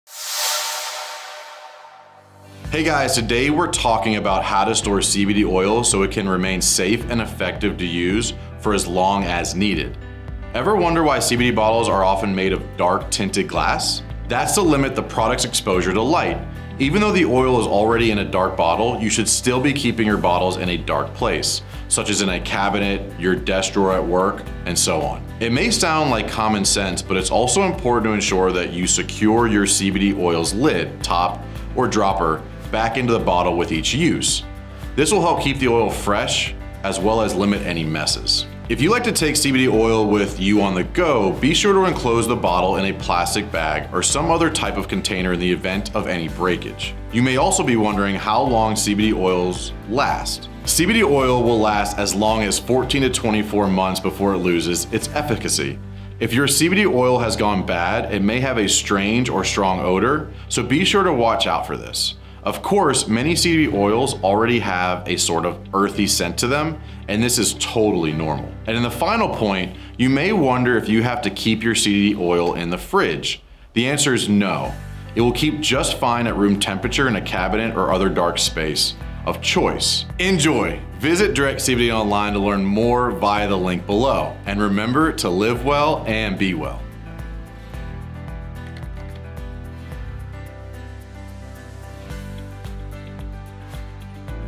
How to Store CBD Oil (& How Long It Lasts)
Storing your CBD oil properly can help it stay fresher — and working at its best — for as long you need. You may also notice that CBD oil and CBD products are often in dark bottles. This is also an important design element to keep your CBD oil fresh. We also discuss how to know if your CBD oil is still "good" and safe to use. Learn more on our blog!